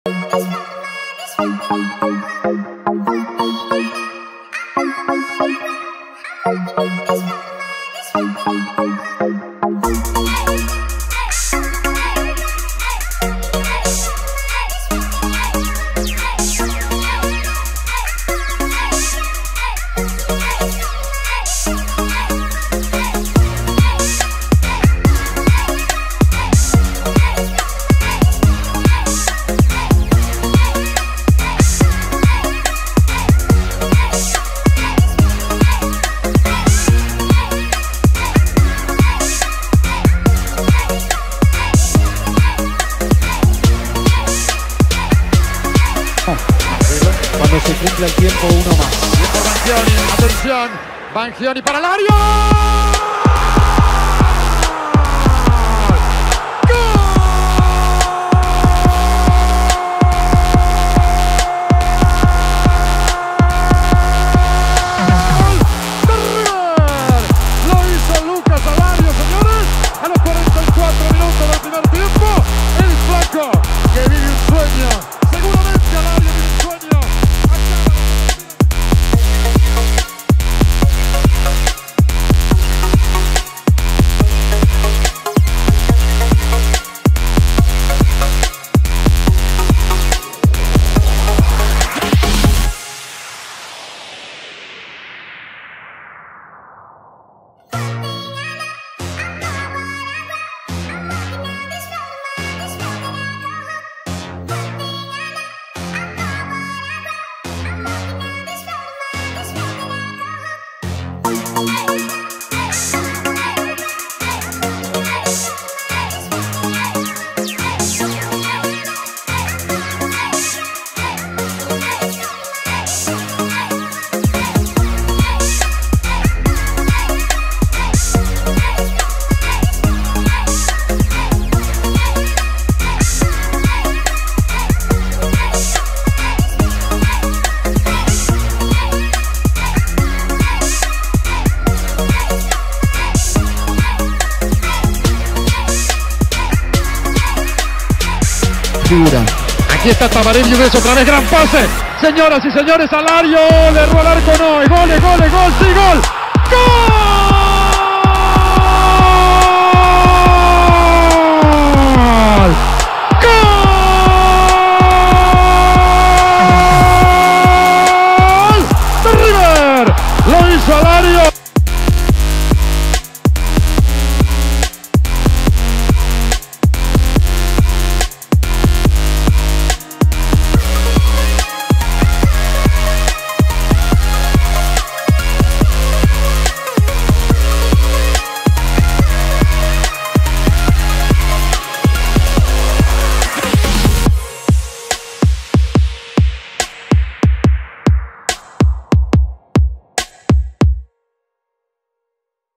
Lucas Alario - River Plate - 2015 HD
Dedicated to my friend Jose!
Lucas Alario Goles en River Plate
Lucas alario all goals in River Plate 2015 - El pipa. El flaco!